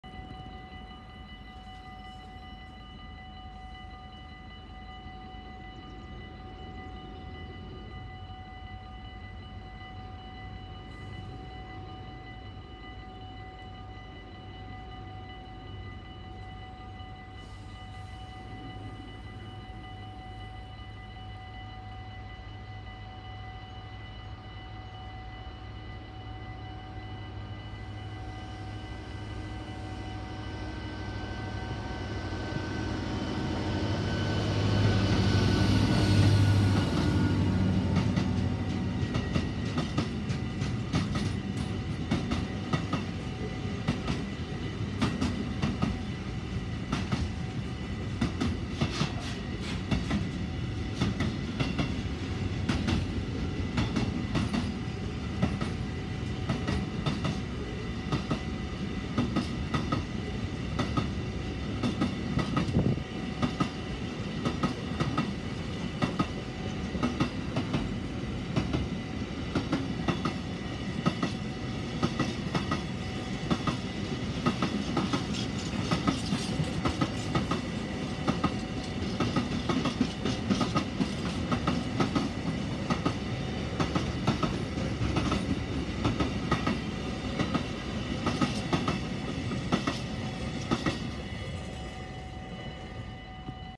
Diesel Locomotive G2000BB RTB-Cargo With Blerick Shuttle Container Train at Blerick the Netherlands
Trainspotting at Blerick the Netherlands, February 16-2023 ! Railfan Video!